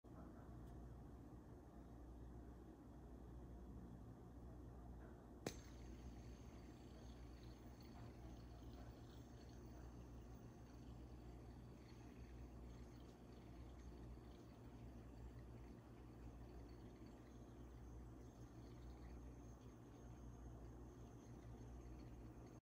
ASMR Video | Multicolor Snitch Fidget Spinner, Infinity Spinning (No Talking)
I hope you enjoy our content at Modern Satisfying Sounds.